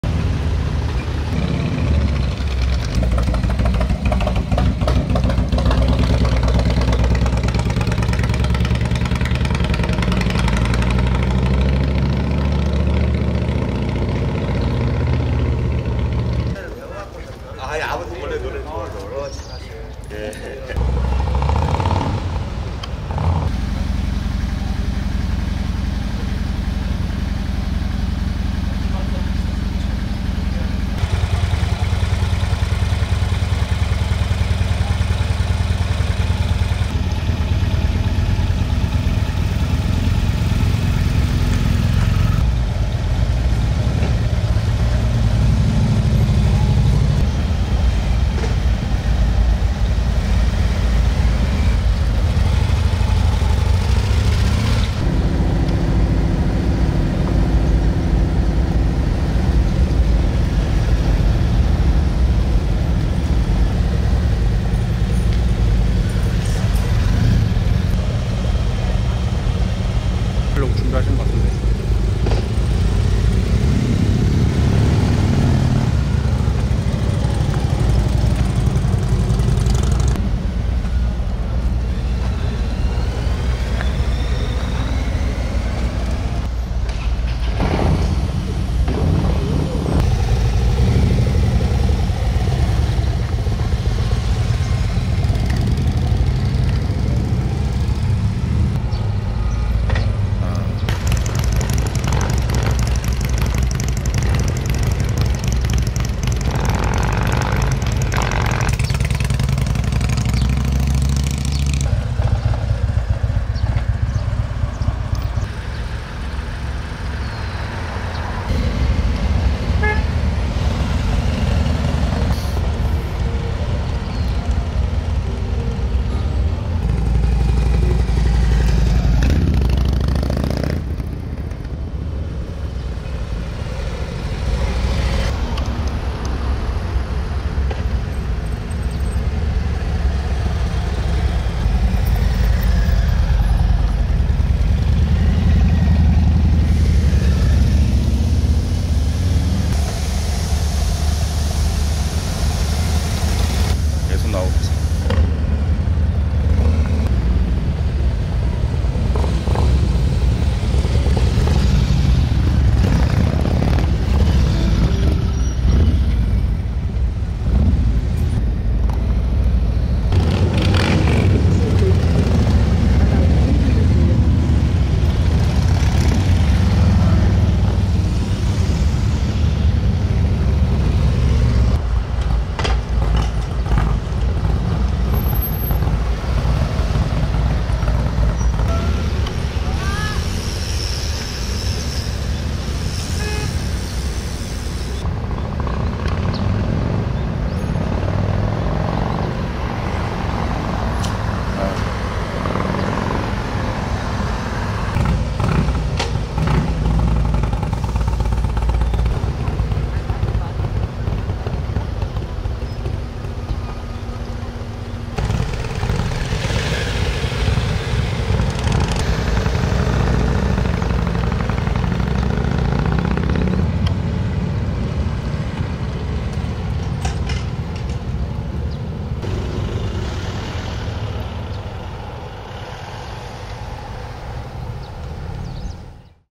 2024 할리데이비슨을 사랑하는 자유인들의 모임
2024 할리데이비슨을 사랑하는 자유인들의 모임 행사 다음날 아침 수안보 온천 풍경을 담아봅니다.